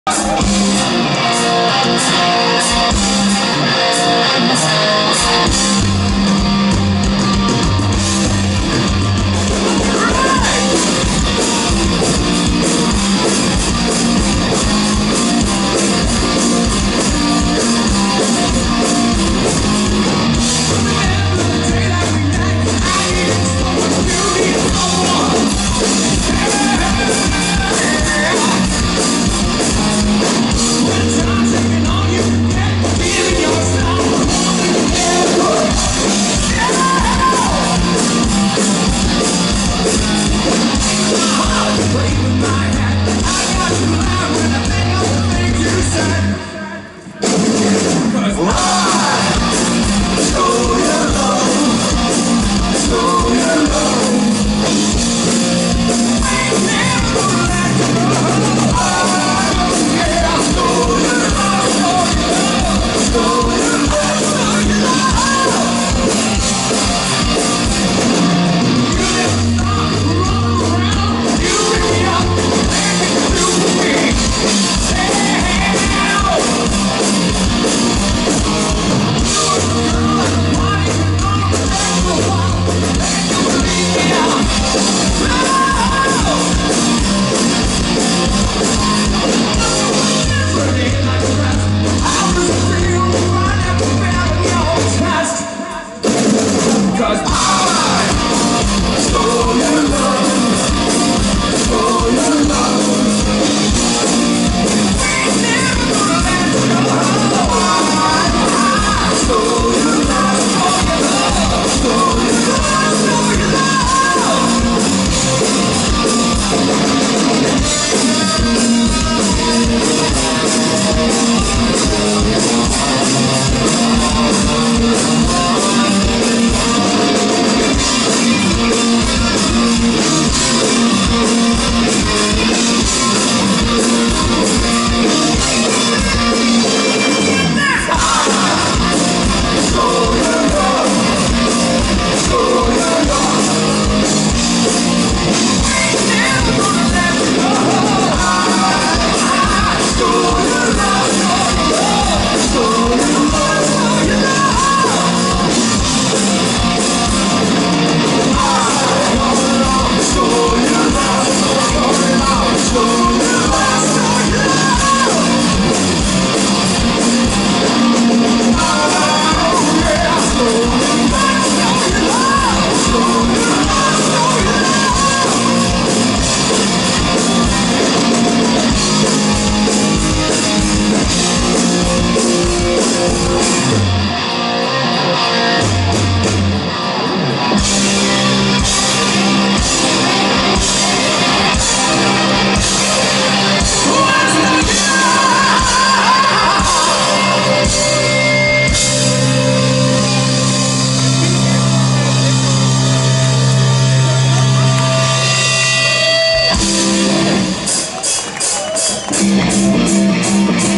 Taste of the Kingsway Festival 2019 Kiss tribute Destroyer
Toronto street festival held each year around Royal York and Bloor. known for tribute bands. here we have Kiss cover band Destroyer. look at the little kid doing the Gene Simmons tongue routine at 2 minutes 40 seconds.MAH03542 2